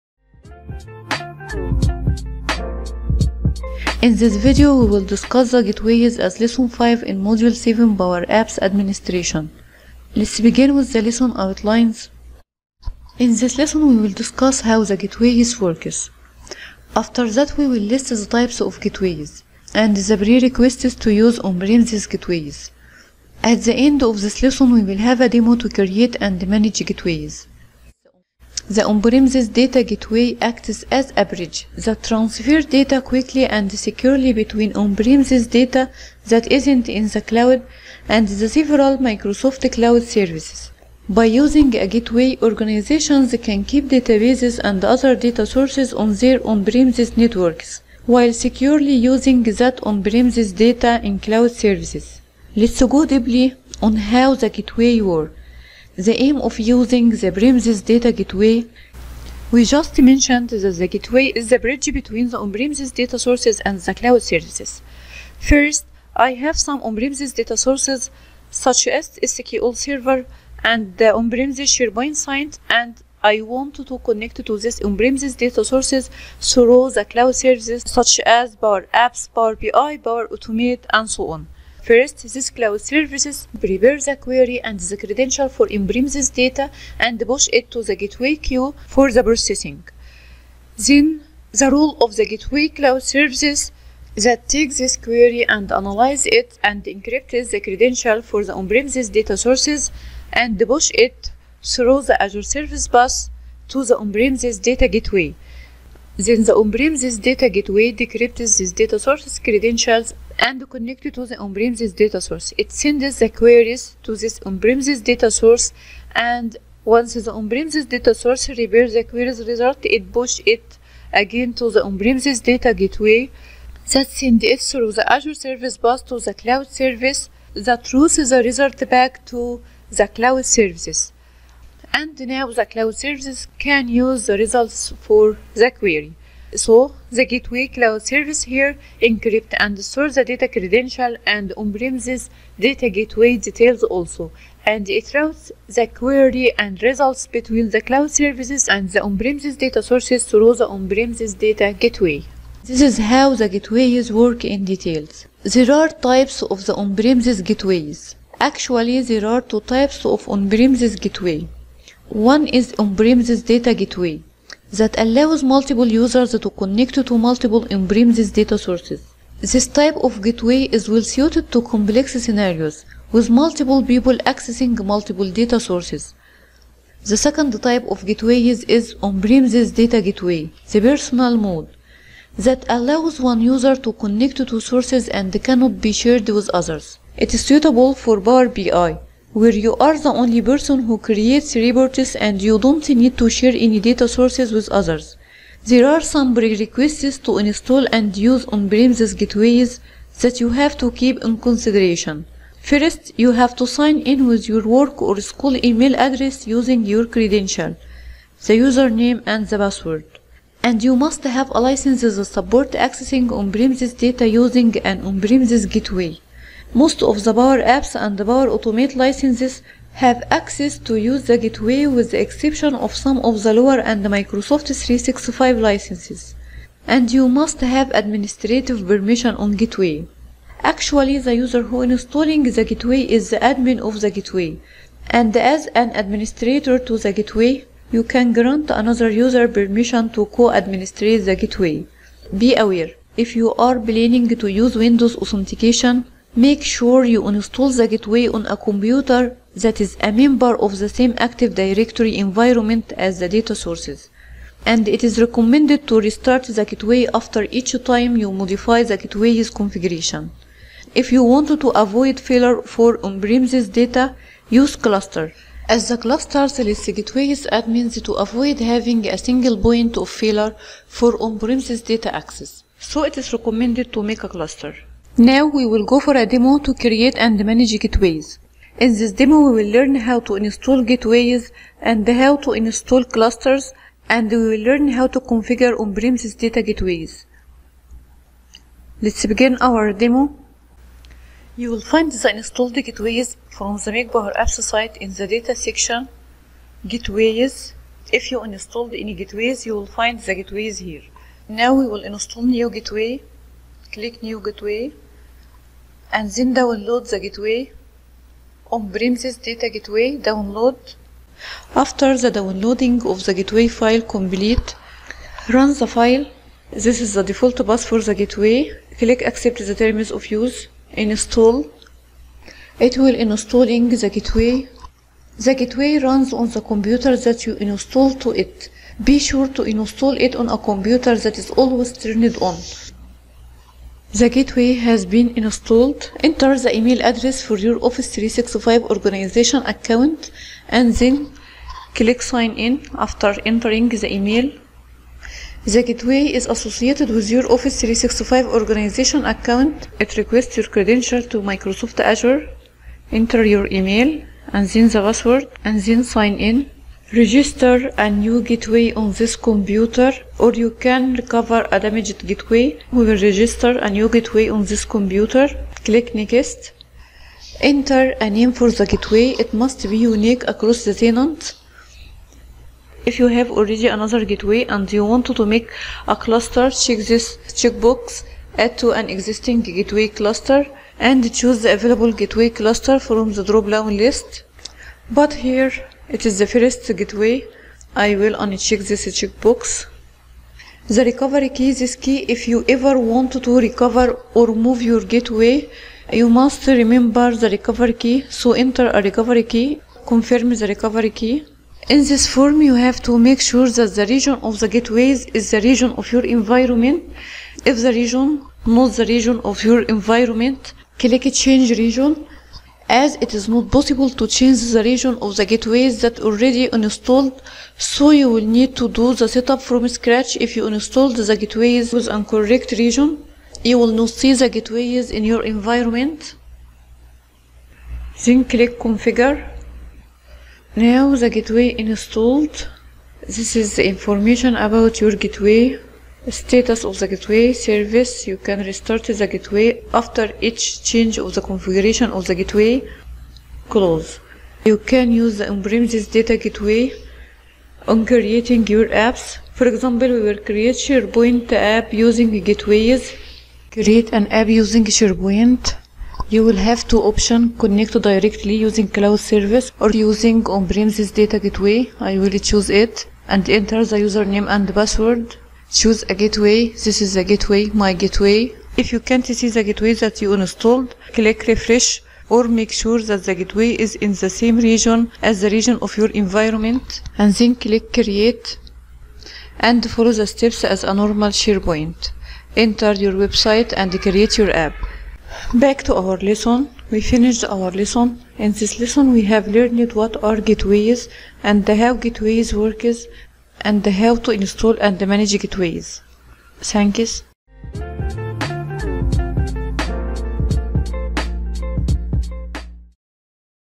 How to install and configure PowerApps Gateways Correctly? - avoid gateways not showning
how ti install and configure power apps gateways?
, and we will discussed  why your installed  gate ways not showing,
what are the powerapps gateway requirements ?
we will discuss how the gateways work?
• we will list the types of gateways? And the prerequisites to to use on-premises data gateway
• At the end we will have a demo to create and manage gateways
The on-premises data gateway acts as a bridge
 that Transfer data quickly and securely between on-premises data (that isn't in the cloud)
and several Microsoft cloud services
• There are some prerequisites to install and use on-premises gateways that you have to keep in consideration
• You must have A license that supports accessing on-premises data using an on-premises gateway. Most of the Power Apps and Power Automate licenses have access to use the gateway with the exception of some of the lower end Microsoft 365 licenses